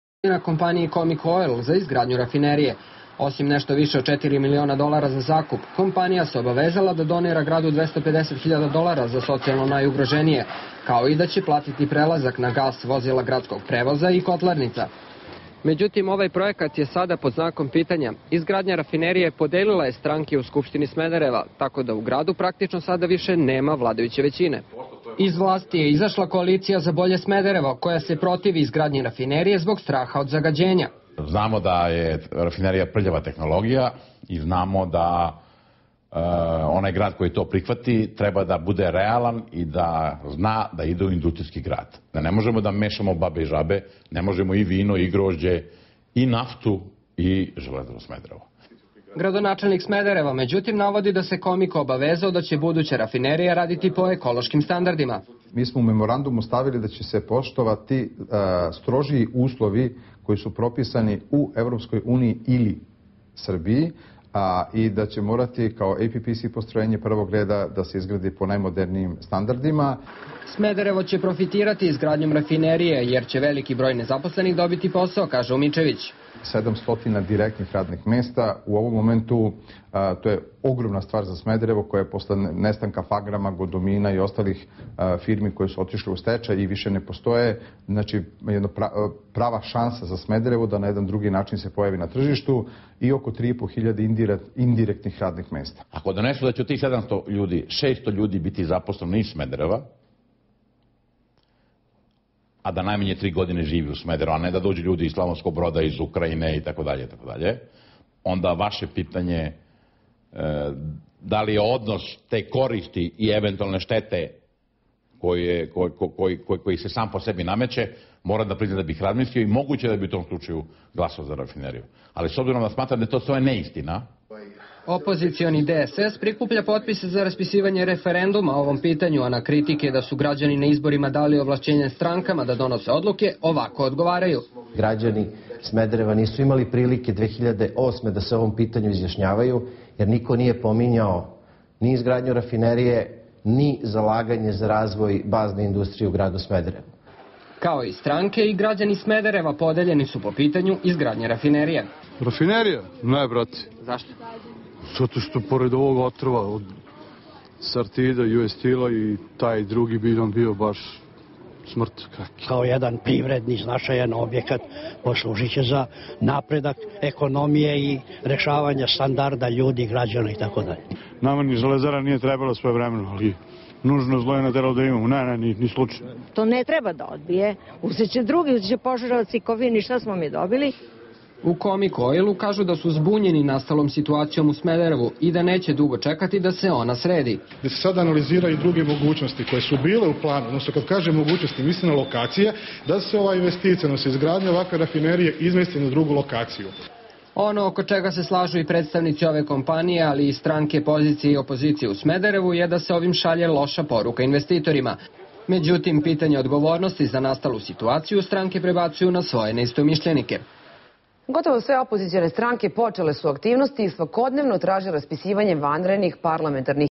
Neizvesna izgradnja rafinerije
Investicija holandskog konzorcijuma koji treba da izgradi rafineriju u Smederevu, dovedena je u pitanje, zbog pitanja oko zaštite životne sredine.